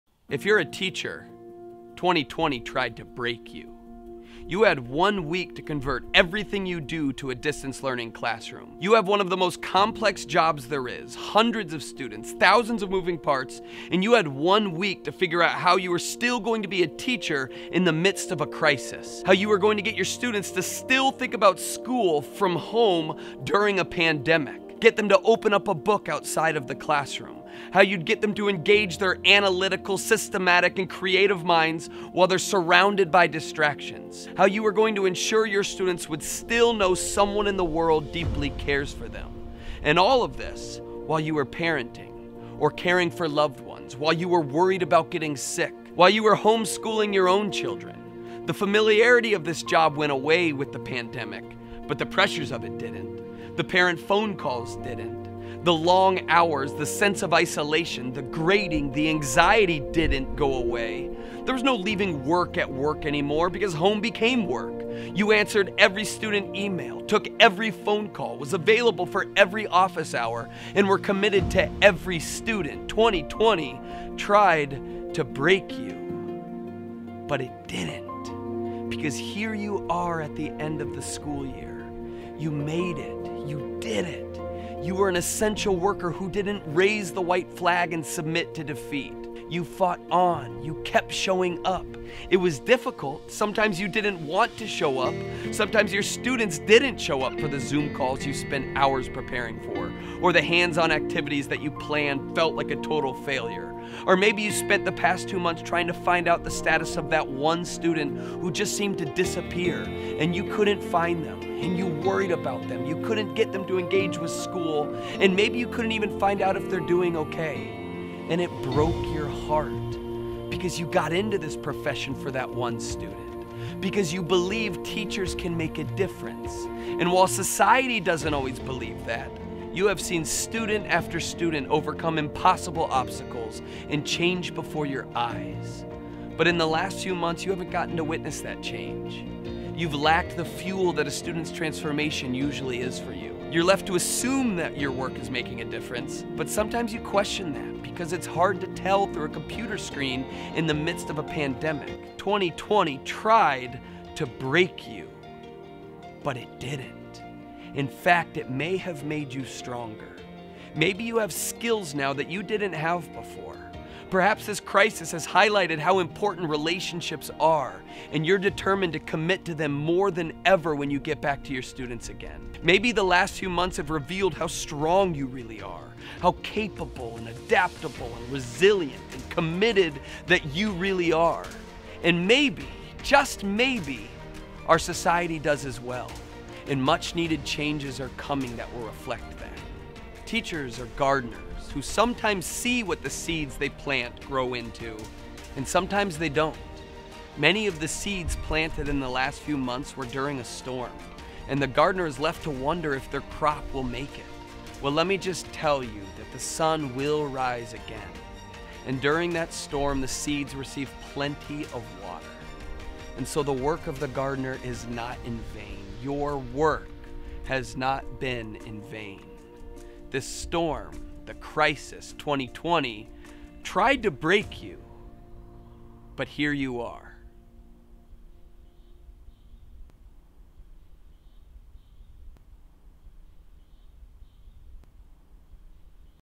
Teachers, 2020 Tried to Break You (But it didn't)
If you are a teacher, 2020 tried to break you.

You had 1 week to convert everything you do to a distance-learning classroom. You have one of the most complex jobs there is, hundreds of students, thousands of moving parts, and you had 1 week to somehow figure out how you were still going to be a teacher in the midst of a crisis. How you were going to get your students to still think about school, from home, during a pandemic. Get them to open up a book outside of the classroom. How you’d get them to engage their analytical, systematic, and creative minds while they are surrounded by distractions. How you were going to ensure your students would still know someone in the world deeply cares for them. And all of this while you were parenting. Or caring for loved ones. While you were worried about getting sick. While you were homeschooling your own children. The familiarity of this job went away with the pandemic.

But the pressures of it didn’t. The parent phone calls didn’t. The long hours, the sense of isolation, the grading, the anxiety didn’t go away. There was no leaving work at work anymore, because home became work. You answered every student email, took every phone call, was available for every office hour, and were committed to every student.

2020 tried to break you. But it didn’t.